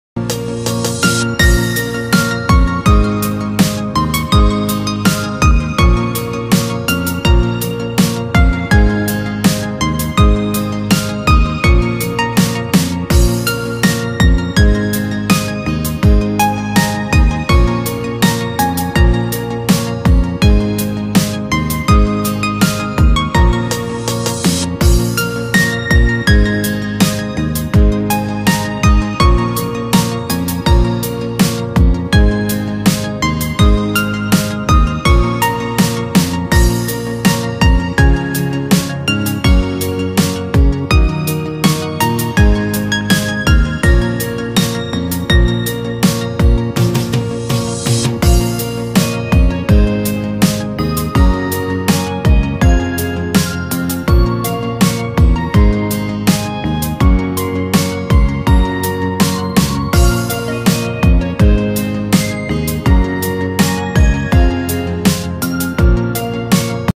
CS06 Mini Body Camera#camera
Product Features:
CS06 Body Camera
HD two million pixels
Uitra-clear infrared night vision
Mobile detection
Noise reduction recording
Hotspot wifi
Type-c interface
Supports loop recording
Rotate 180 degrees
Video resolution:1920*1080P
Battery capacity:1400mAh
APP:Sports Camera